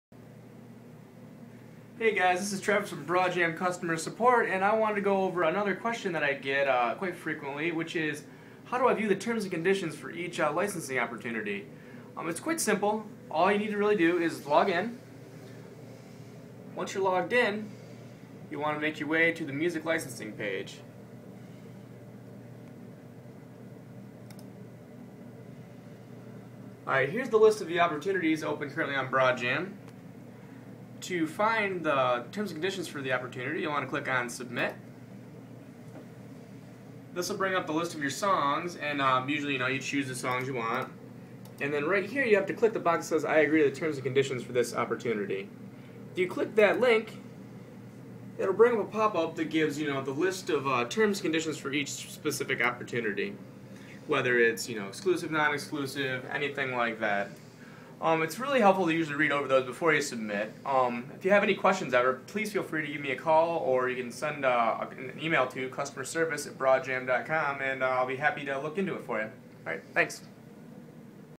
Viewing Terms & Conditions on Broadjam
Travis from Broadjam Customer Support shows you where you can view the Terms & Conditions on each Broadjam opportunity.